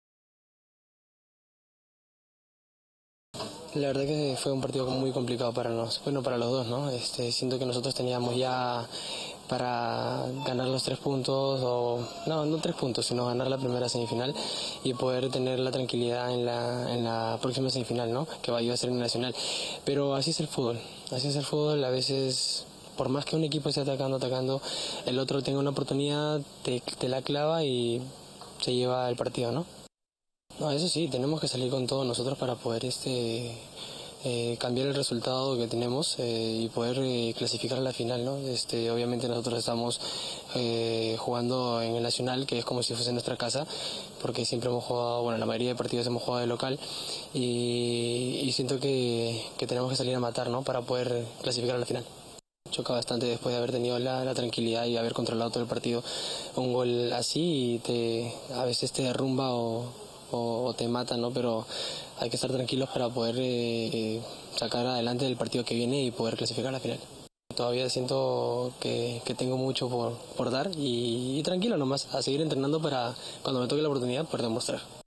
Brandon Palacios: "Por más que un equipo esté atacando, el otro se puede llevar el partido"
Derrota difícil de asimilar. Sporting Cristal cayó en la primera semifinal de la Liga1 Movistar. Los rimenses perdieron sobre la hora con Alianza Lima, a través del gol de Aldair Fuentes. Pese a ser superiores en la tenencia del balón, los 'celestes' no fueron eficaces, pero para Brandon Palacios, juvenil de Cristal, el resultado global es remontable.